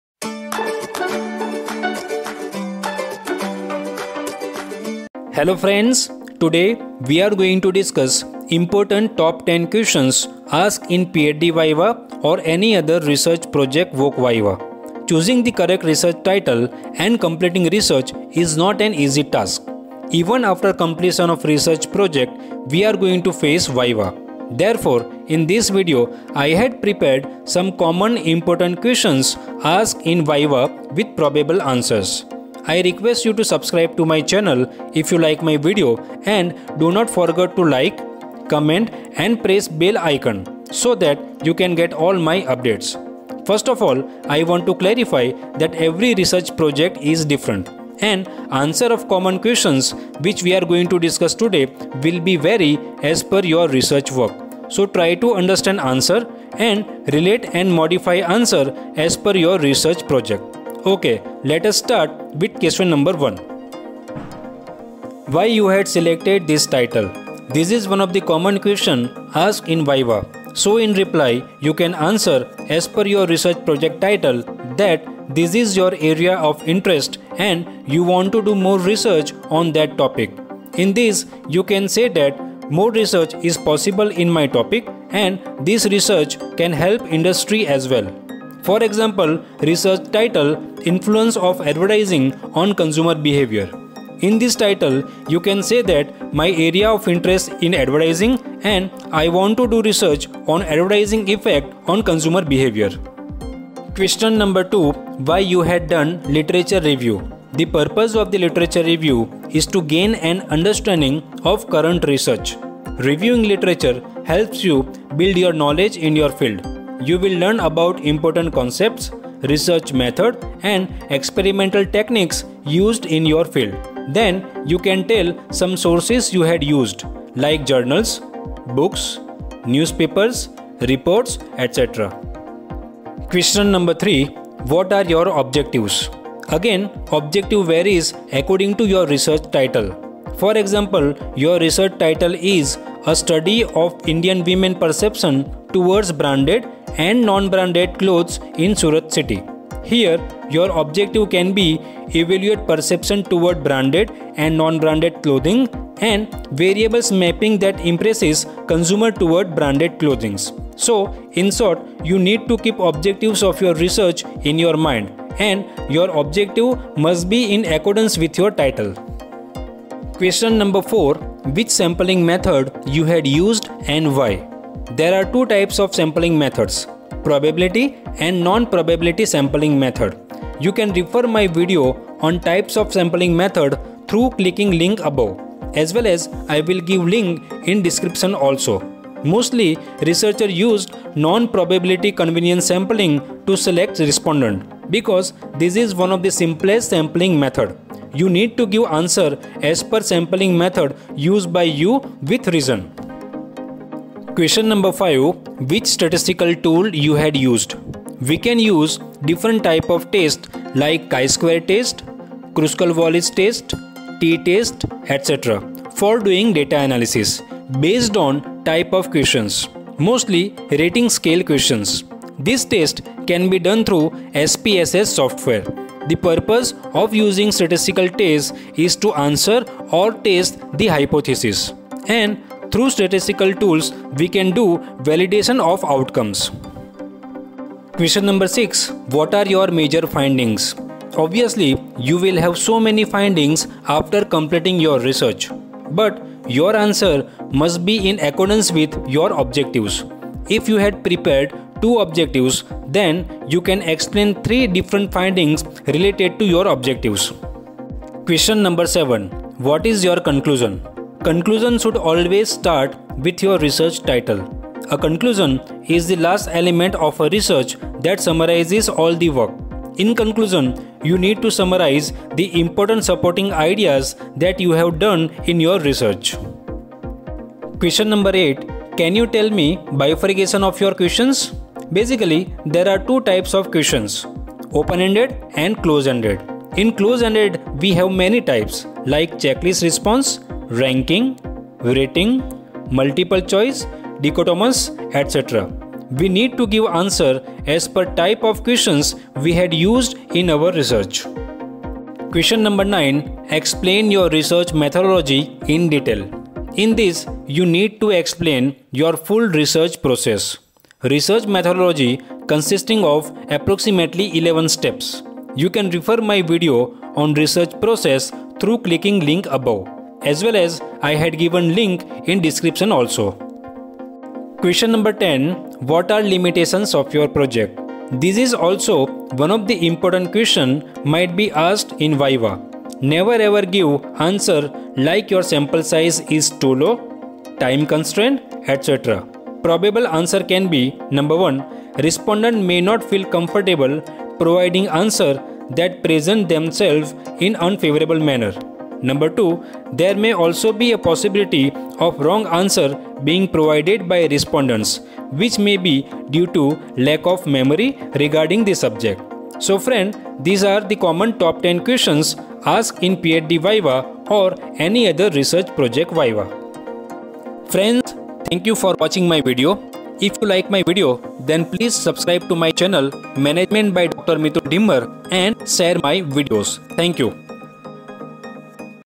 Viva Questions with answer (Top 10) for Research Project, Dissertation and PhD Thesis
Hello friends.
In this video I had discussed basic top 10 questions asked in PhD viva, research project viva.
I had also given probable answer of each questions.
First of all you need to understand that every research project is different.

And answer of common questions which we are going to discuss today will be vary as per your research work.

So try to understand answer and relate and modify answer as per your research project.

I had prepared two videos which will be helpful for you to understand answer in detail.

Following viva questions I had discussed in this video
TIMESTAMPS
0:00 Welcome to my channel Management By Dr. Mitul Dhimar
1:05 1. Why you had selected this title?
1:44 2. Why you had done literature review?
2:13 3. What are your objectives?
2:50 4. Which sampling method you had used and why?
3:24 5. Which statistical tool you had used?
3:57 6. What are your major findings?
4:17 7. What is your conclusion?
4:38 8. Can you tell me bifurcation of your questions?
5:02 9. Explain your research methodology in detail.
5:24 10. What are limitations of your project?

I hope my video helped you in preparation of PhD and other research project viva.

Majority of photographs are downloaded from freepik.com